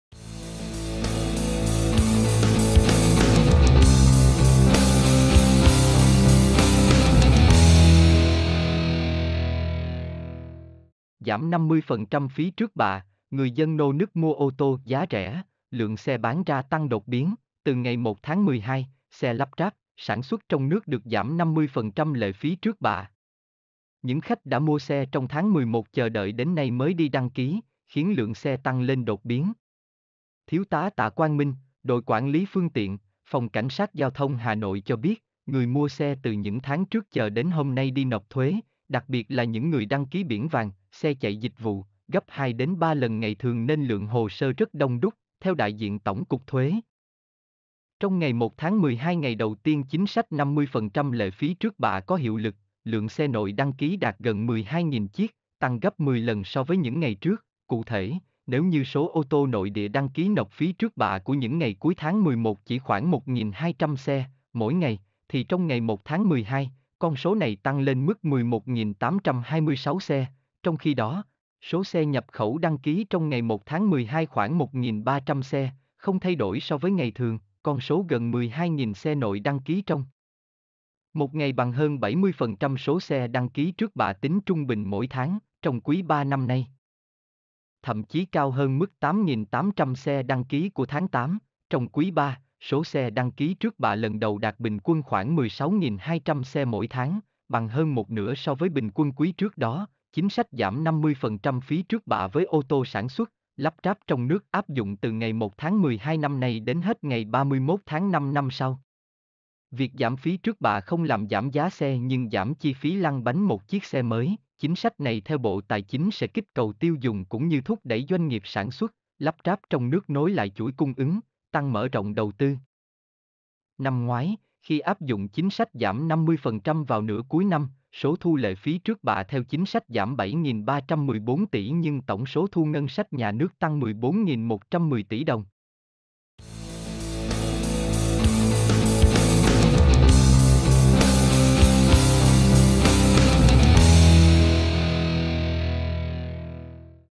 Giảm 50% phí trước bạ, người dân nô nức mua ô tô "giá rẻ", lượng xe bán ra tăng đột biến
Theo đại diện Tổng cục Thuế, trong ngày 1/12 - ngày đầu tiên chính sách 50% lệ phí trước bạ có hiệu lực, lượng xe nội đăng ký đạt gần 12.000 chiếc, tăng gấp 10 lần so với những ngày trước.

Cụ thể, nếu như số ôtô nội địa đăng ký nộp phí trước bạ của những ngày cuối tháng 11 chỉ khoảng 1.200 xe (mỗi ngày), thì trong ngày 1/12, con số này tăng lên mức 11.826 xe.

Trong khi đó, số xe nhập khẩu đăng ký trong ngày 1/12 khoảng 1.300 xe, không thay đổi so với ngày thường.

Con số gần 12.000 xe nội đăng ký trong một ngày bằng hơn 70% số xe đăng ký trước bạ tính trung bình mỗi tháng (trong quý III năm nay), thậm chí cao hơn mức 8.800 xe đăng ký của tháng 8.

Trong quý III, số xe đăng ký trước bạ lần đầu đạt bình quân khoảng 16.200 xe mỗi tháng, bằng hơn một nửa so với bình quân quý trước đó.

Chính sách giảm 50% phí trước bạ với ôtô sản xuất, lắp ráp trong nước áp dụng từ 1/12 năm nay đến hết 31/5 năm sau. Việc giảm phí trước bạ không làm giảm giá xe nhưng giảm chi phí lăn bánh một chiếc xe mới.

Chính sách này theo Bộ Tài chính sẽ kích cầu tiêu dùng cũng như thúc đẩy doanh nghiệp sản xuất, lắp ráp trong nước nối lại chuỗi cung ứng, tăng mở rộng đầu tư. Năm ngoái, khi áp dụng chính sách giảm 50% vào nửa cuối năm, số thu lệ phí trước bạ theo chính sách giảm 7.314 tỷ nhưng tổng số thu ngân sách nhà nước tăng 14.110 tỷ đồng.